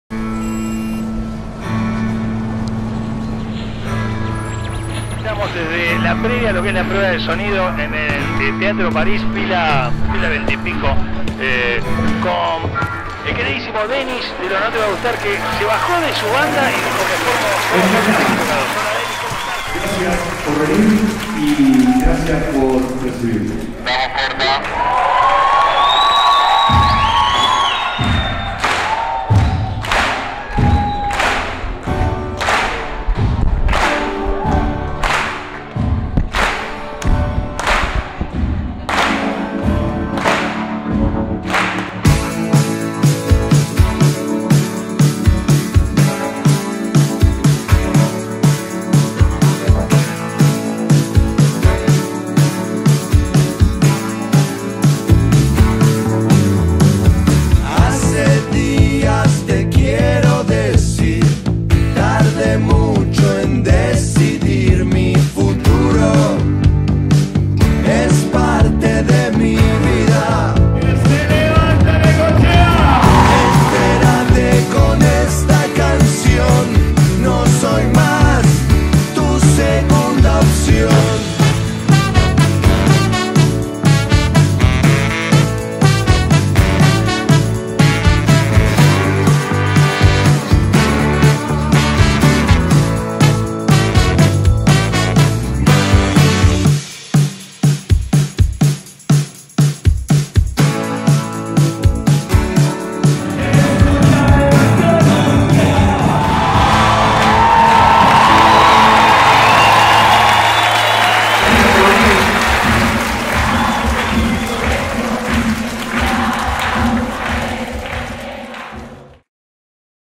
NoTe Va Gustar en Necochea
Esto paso el viernes 29 de noviembre con la visita de @notevagustaroficial a la ciudad de Necochea!
Gracias ciudad por acompañarnos!

Produccion: K2 Media